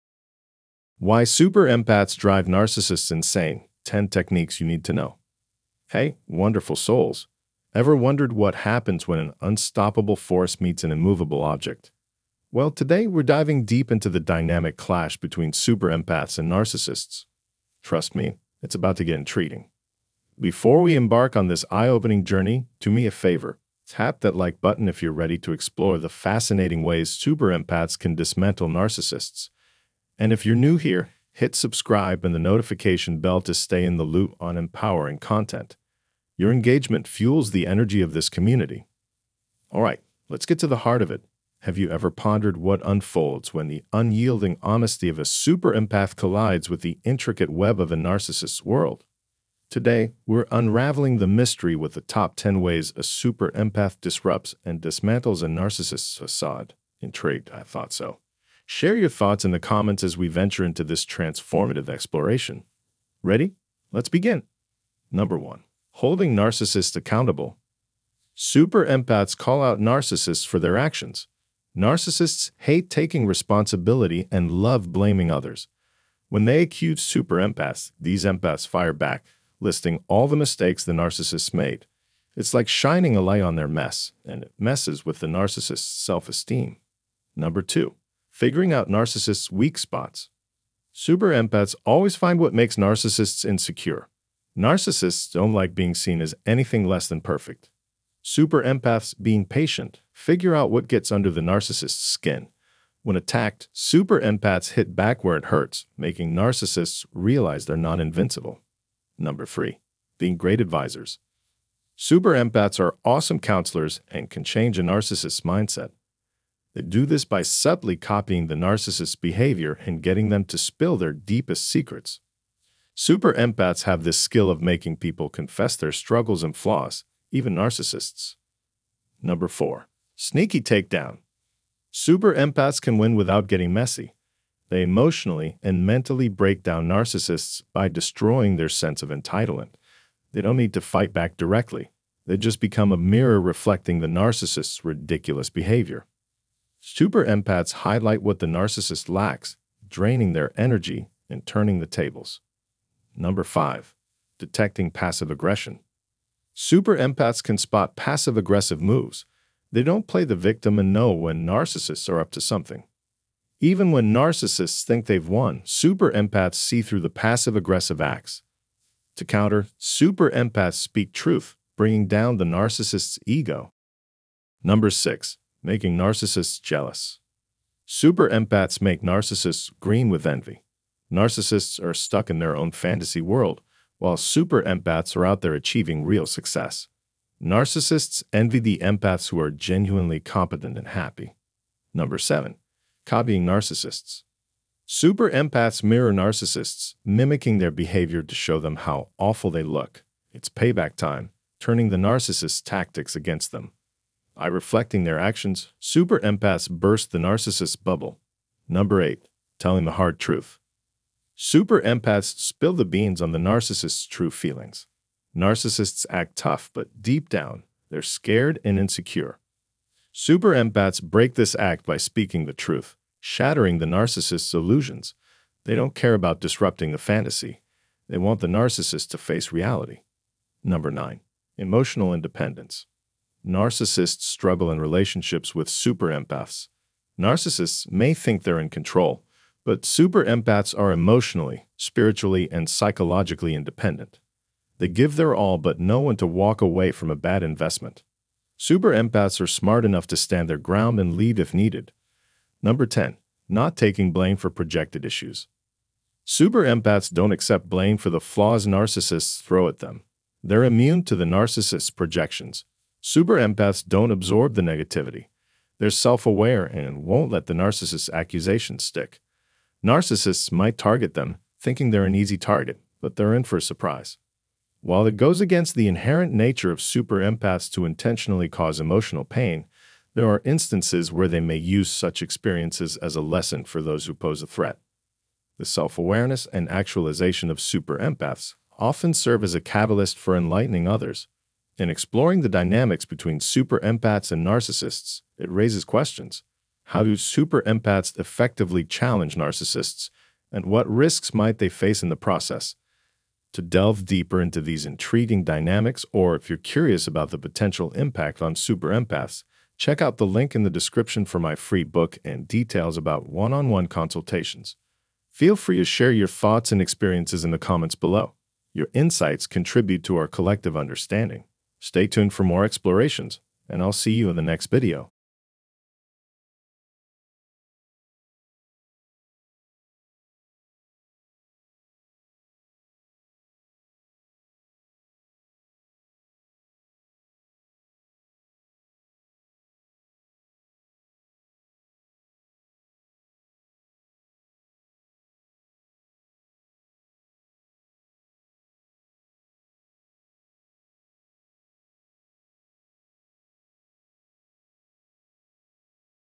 Why Super Empaths Drive Narcissists Insane - 10 Techniques You Need to Know | NPD | Healing
Why Super Empaths Drive Narcissists Insane - 10 Techniques You Need to Know | NPD | Healing | Empaths Refuge

Have you ever been in a situation where you just didn't understand where someone was coming from? Maybe you couldn't relate to their emotions or perspective, and as a result, you struggled to connect with them. It can be tough to navigate complex social situations, but that's where empathy comes in.

Empathy is a crucial component of emotional intelligence - it's the ability to not only feel what others feel, but also to truly understand their point of view. When we practice empathy, we can soothe a friend who's going through a tough time, or resolve conflicts with coworkers by truly listening and understanding their needs.